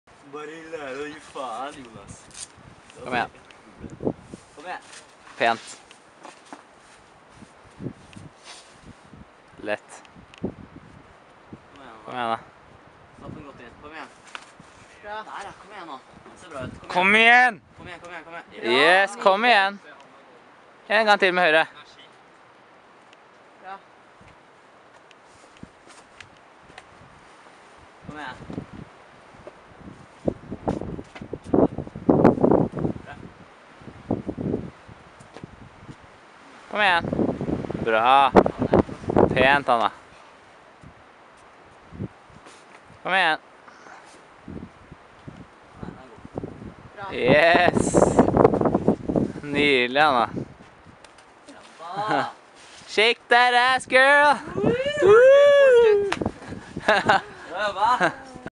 Malcoms egg 7B+
Hannah sends Malcoms egg 7B+ in sandefjord-Norway